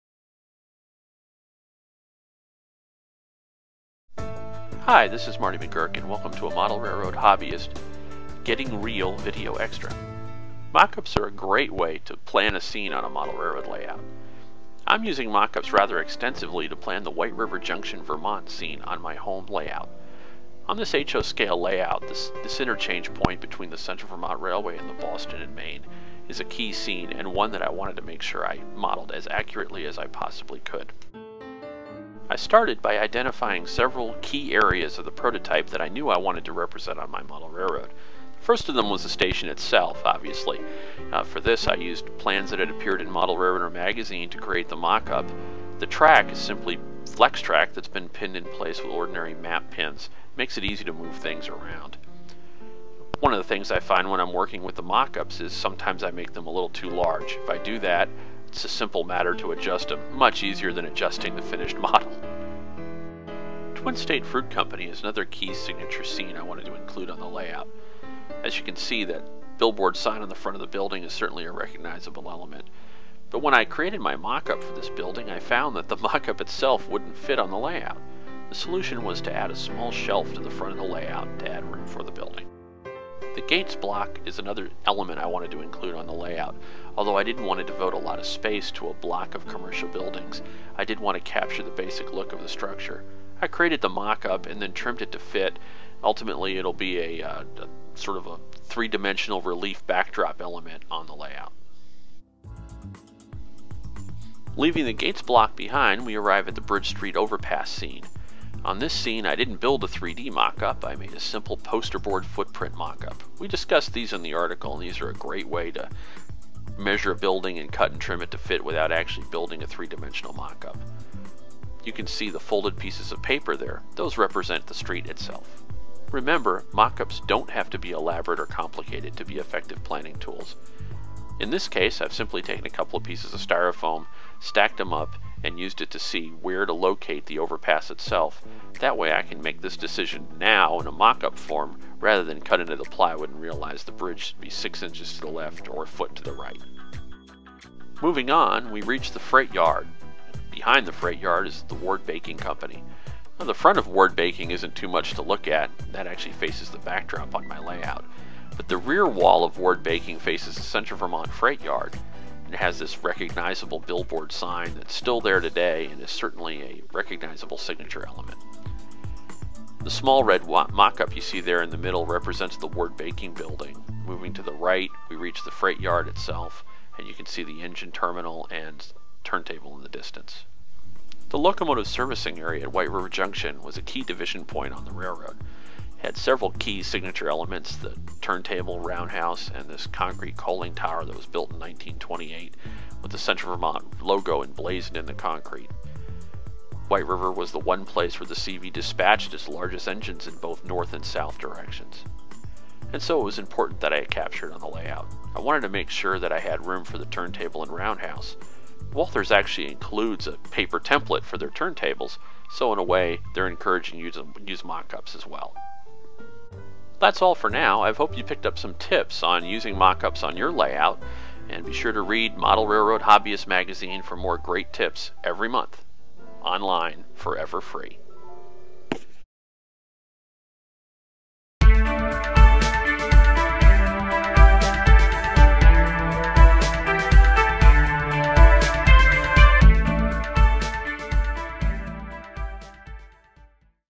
Mocking up a Junction
Marty McGuirk demonstrates how used mockups on his layout to plan prototype-inspired scenes. The video is a companion to the March 2012 issue of Model Railroad Hobbyist magazine